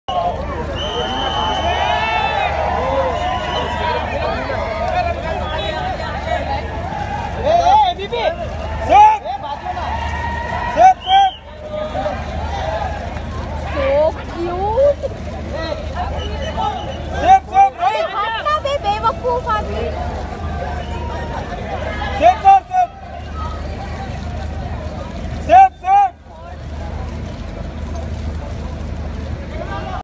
Kareena Kapoor,Karisma Kapoor, Saif Ali Khan with Baby Taimur Arrive for Sonam Kapoor’s Wedding
Sonam Kapoor- Anand Ahuja Marriage.